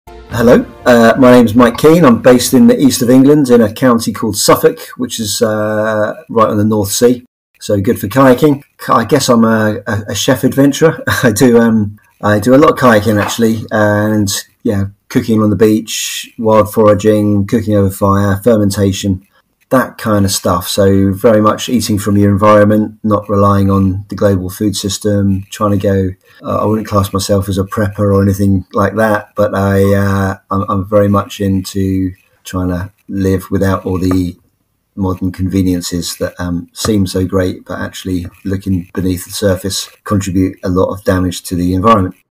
Chef, Kayaker, and Adventurer Mike Keen: A Greenland Expedition and Sustainable Adventure Made by He
We are joined by chef and adventurer @Eatyourenvironment to talk about food, sustainable living and adventure, and his upcoming expedition to Greenland!